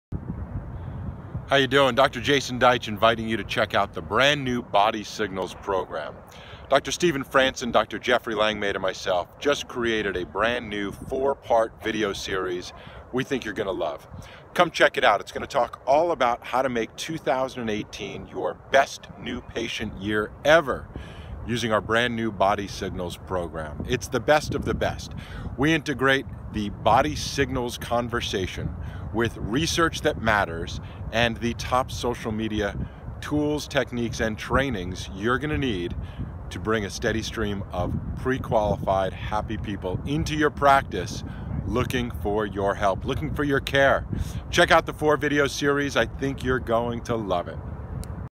The Best New Patient Year Ever
Make 2018 the best new patient year you've ever had. We're introducing a program to help you bring in qualified, informed patients into your practice. Don't wait, check it out now at  bodysignalsprogram.com